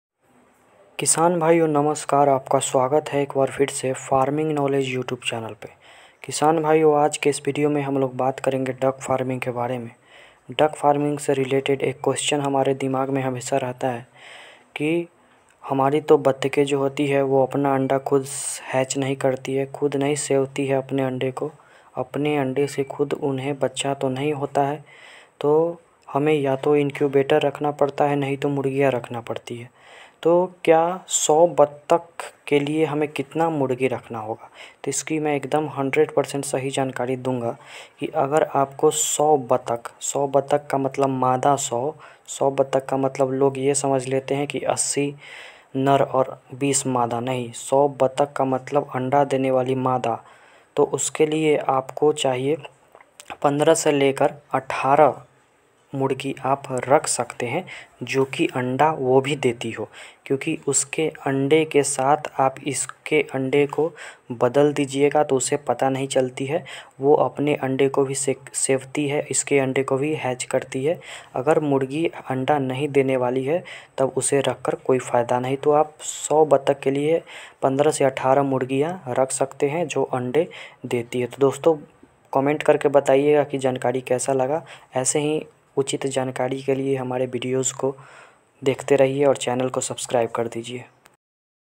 100 बत्तख के अंडे को hatch करने के लिए कितने मुर्गी रखना होगा ।। Duck Farming ।।
Duck Farming दोस्तों नमस्कार आपका स्वागत है फार्मिंग नॉलेज युटुब चैनल पर दोस्तों आज के इस वीडियो में जैसे कि मैंने बताया कि 100 बत्तख के अंडे को हैक करने के लिए हमें कितनी मुर्गियों की जरूरत होती है तो हमें 18 से लेकर 15 मुर्गियों की जरूरत होती है तो वीडियो अच्छा लगा तो चैनल को भी कर दीजिए सब्सक्राइब वीडियो को अभी कर दीजिए लाइक हम प्रतिदिन आपके लिए फार्मिंग से रिलेटेड वीडियोस लाते रहेंगे